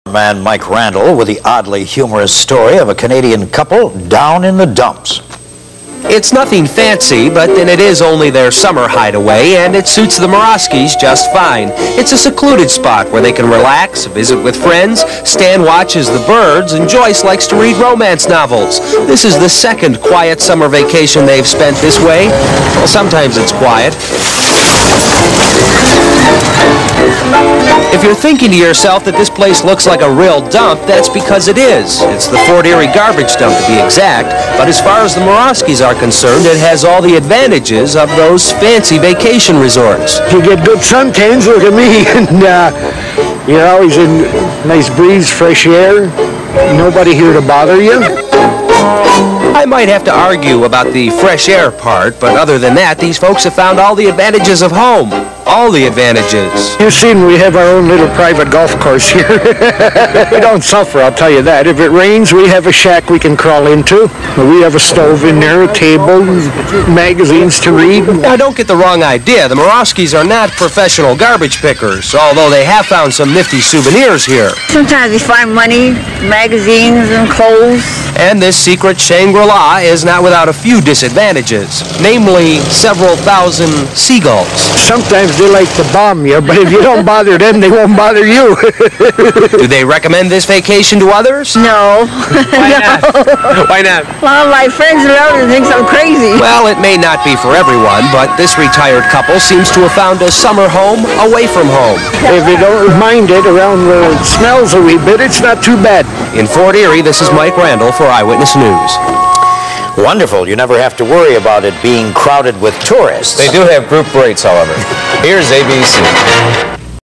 Mike Randall Feature-Garbage Get-A-Way 1986
Interesting couple that liked to vacation in the garbage dump. seriously.